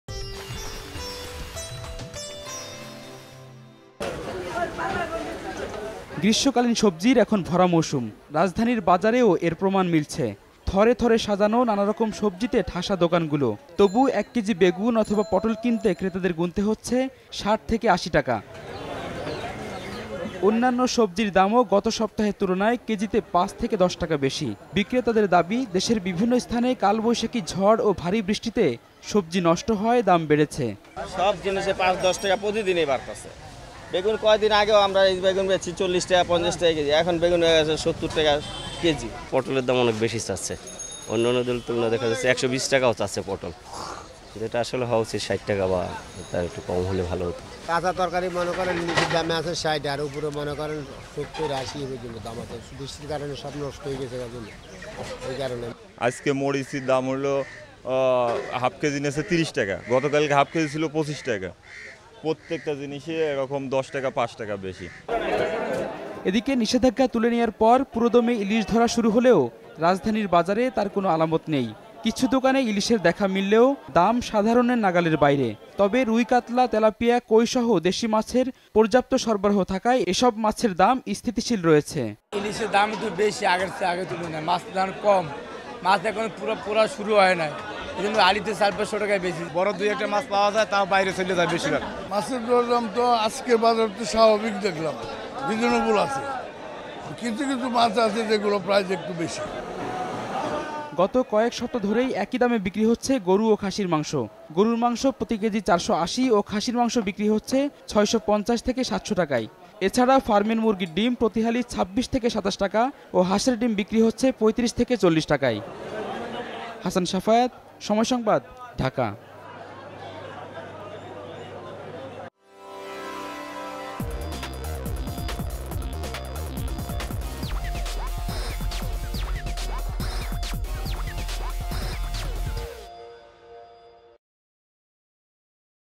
সবজির দাম বেড়েছে ৫ থেকে ১০ টাকা | Business News | Market Price
ভরা মৌসুমে কোনো ঘাটতি না থাকলেও চলতি সপ্তাহে ৫ থেকে ১০ টাকা পর্যন্ত বেড়েছে সব ধরনের সবজির দাম। এদিকে, নিষেধাজ্ঞা উঠে গেলেও রাজধানীর বেশিরভাগ বাজারেই ইলিশের সরবরাহ কম। এছাড়া সব ধরনের মাংস ও ডিমের দাম স্থিতিশীল আছে।